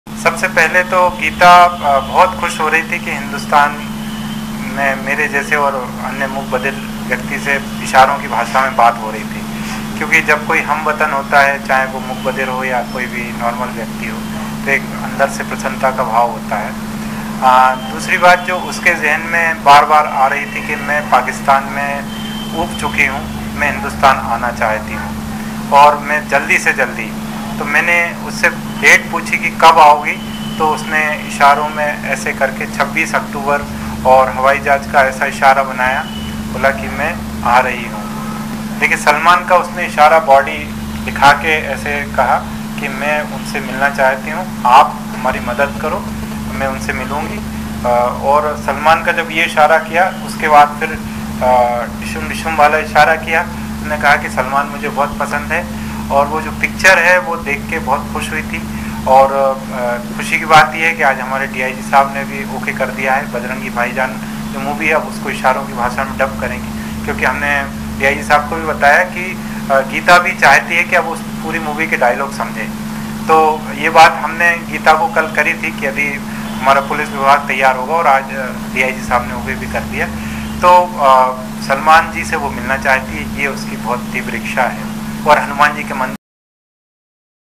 सलमान खान से मिलना चाहती है गीता
गलती से सीमा लांघ कर पाकिस्तान पहुंची मूक-बधिर गीता भारत लौटने के बाद अपने परिवार के साथ ही सलमान खान से भी मिलना चाहती है। कराची शहर में रह रही गीता ने इंटरनेट पर वीडियो कॉल के जरिए सांकेतिक भाषा विशेषज्ञ ज्ञानेंद्र पुरोहित से यह इच्छा जताई है।